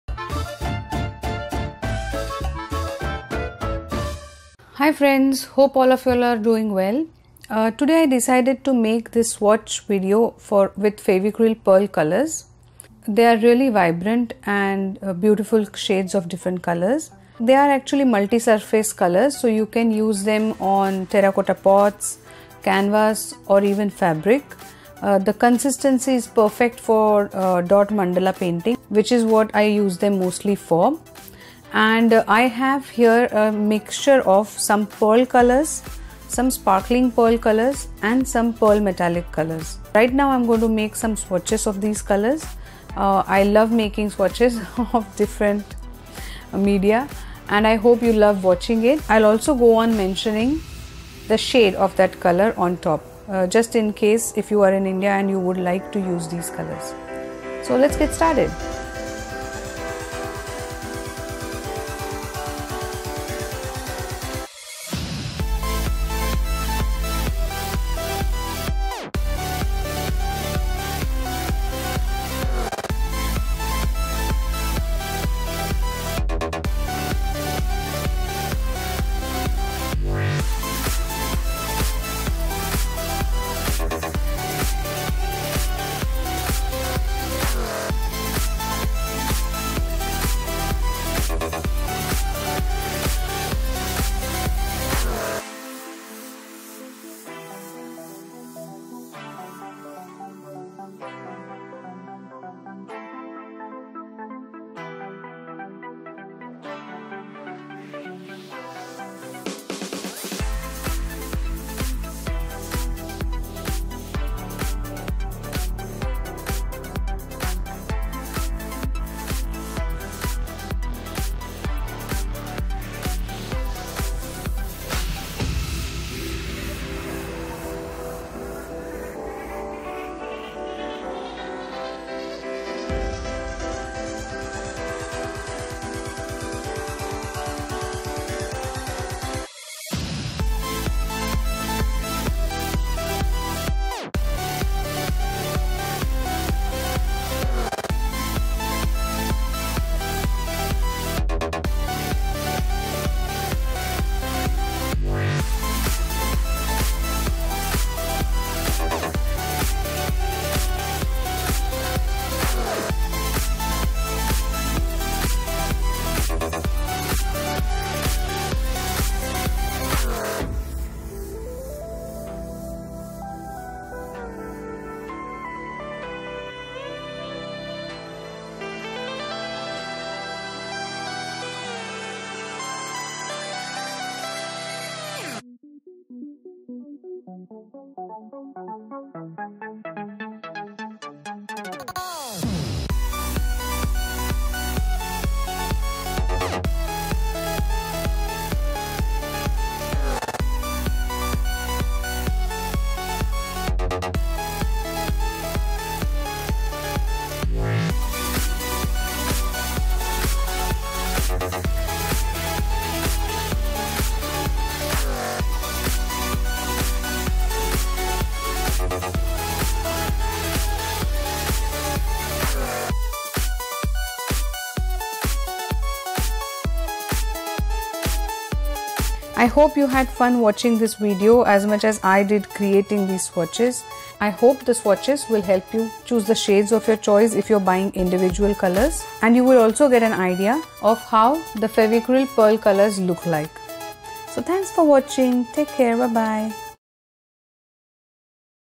Fevicryl hobby Pearl Colours swatches
Hi friends!! Enjoy these Fevicryl hobby Pearl Colours swatches!! these colours are vibrant, super sparkly and shiny and can be used in many surfaces!!